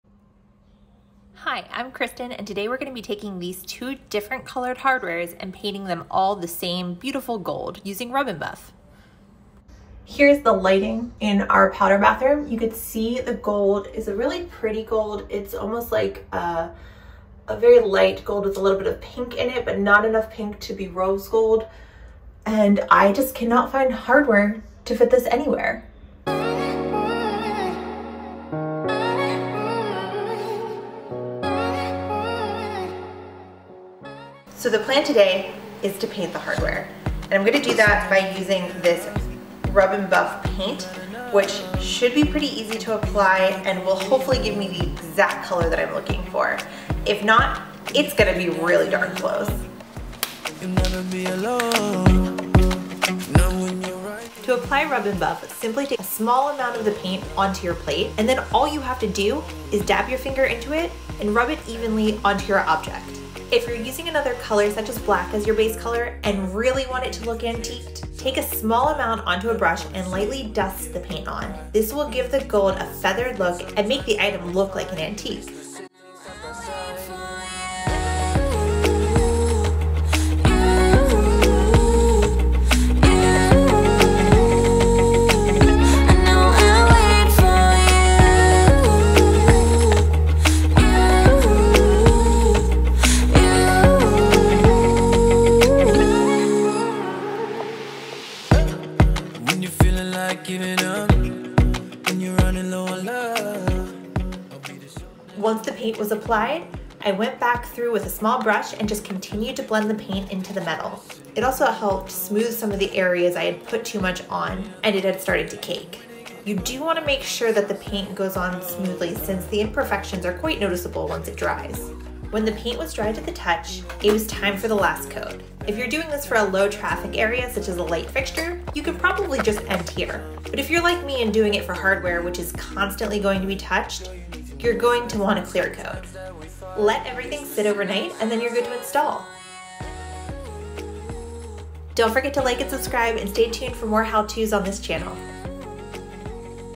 Love Gold Accents? How to use Rub n Buff to Update your Look
Like your hardware, but don't like the color? Want to update your lamp to a trendy gold? Rather than buying new, DIY it and learn how to use rub n buff!

Tools you’ll need:
* Your hardware (or lamp, bed frame, really anything you plan to use it with)
* Rub n Buff – I’m using the color Antique Gold
* Latex gloves
* A cloth
* A paper plate
* A small brush – useful but not necessary